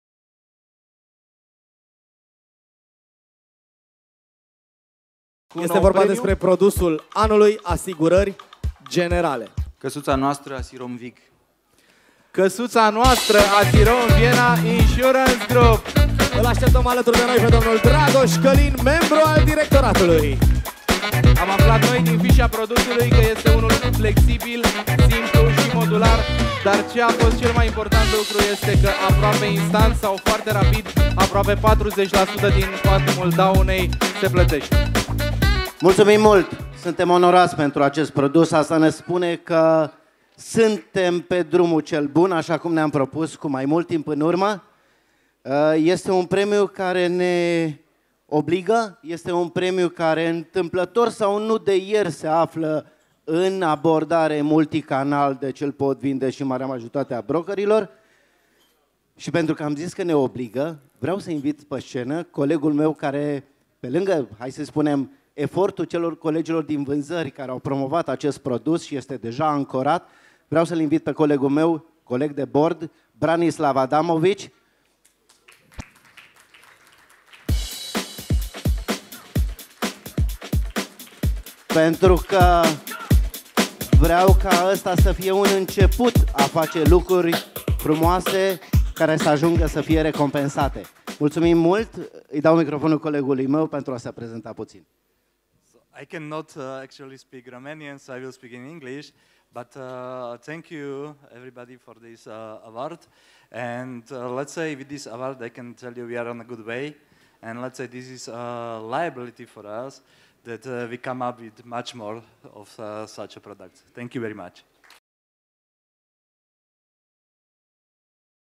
Produsul Anului Asigurari Generale Dragos CALIN Membru Directorat, ASIROM VIG Branislav ADAMOVIC
Gala Premiilor Pietei Asigurarilor Editia 2015. Video original aici